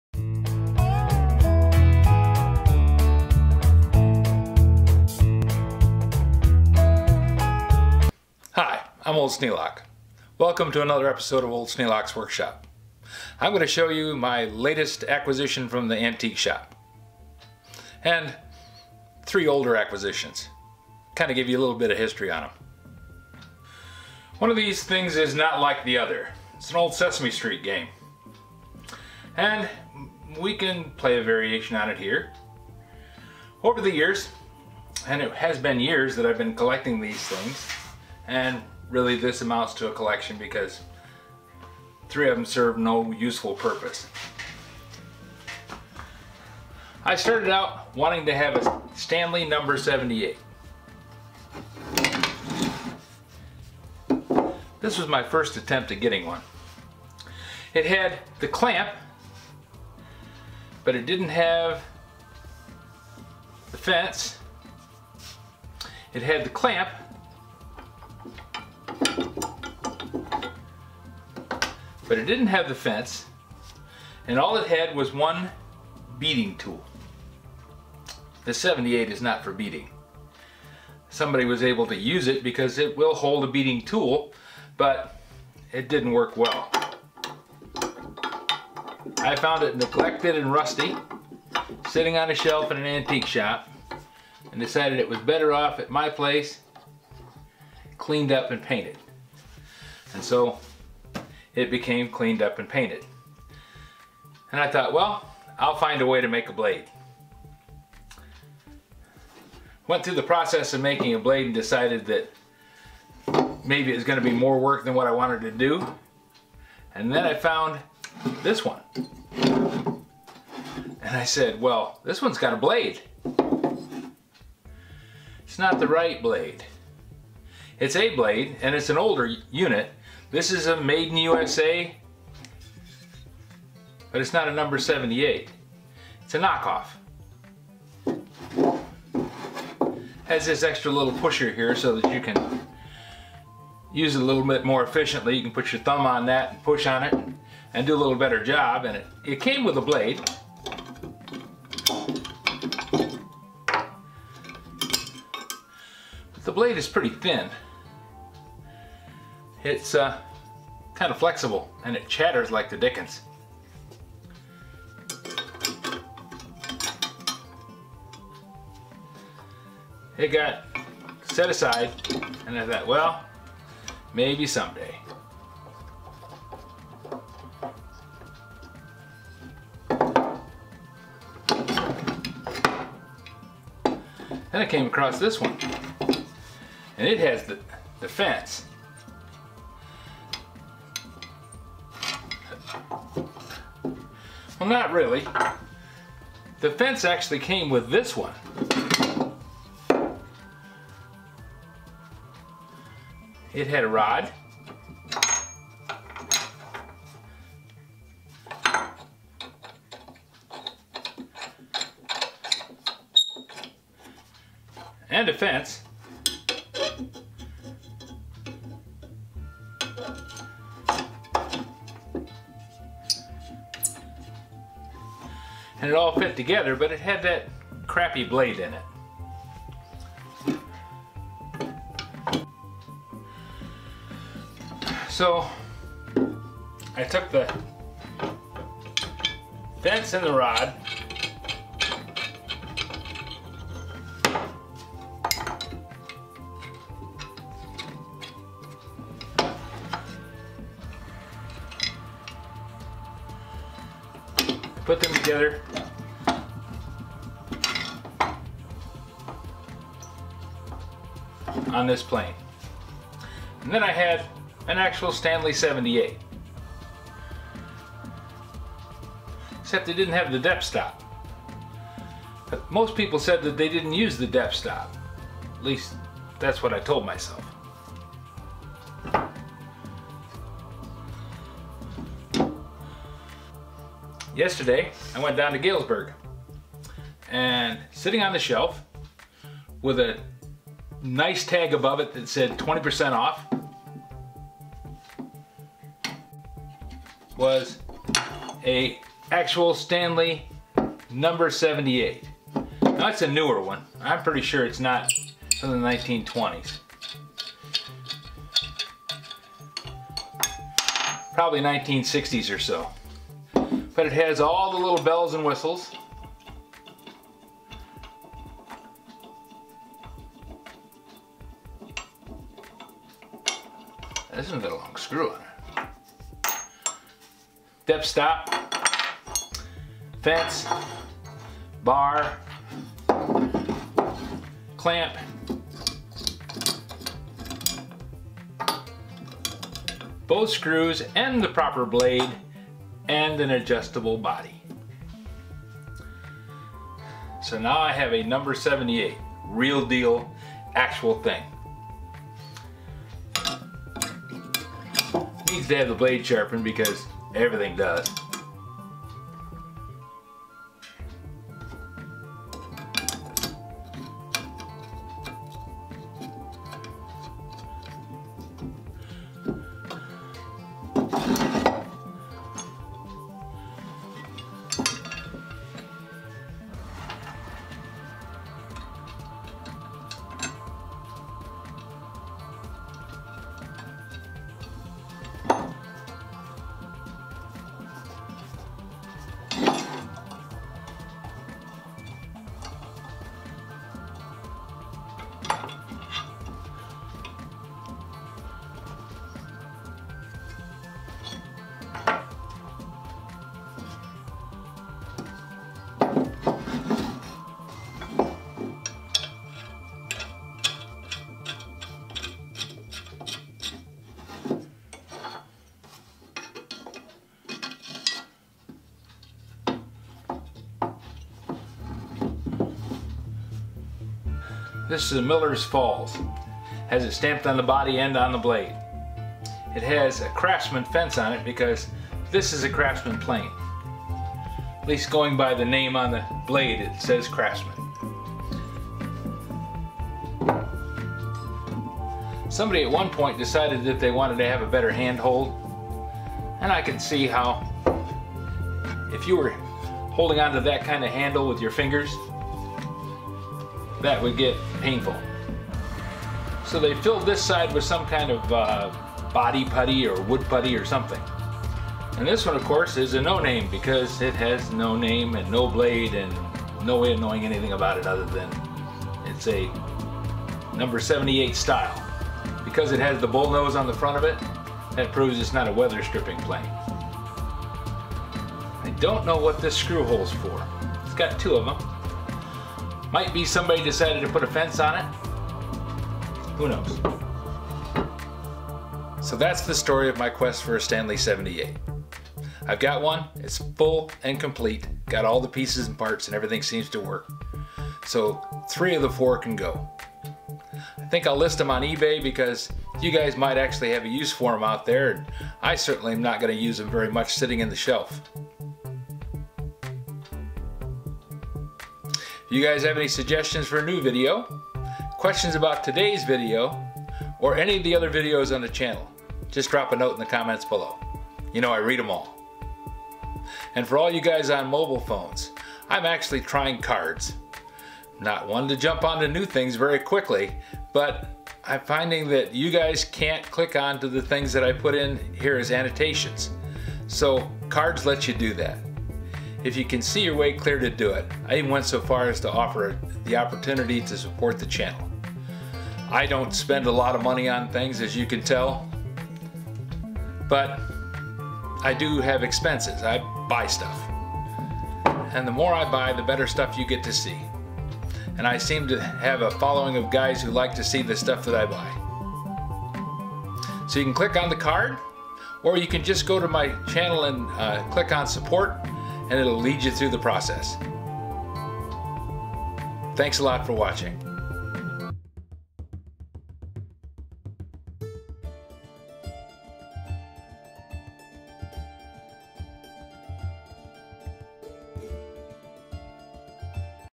Antique Shop Finds The Stanley #78 and Clones ~ Old Sneelock's Workshop
Another long search ended. Years after I thought I had a use for a Stanley #78 plane I finally have a complete one on the bench. I’ll walk you through the hunt and show you the result.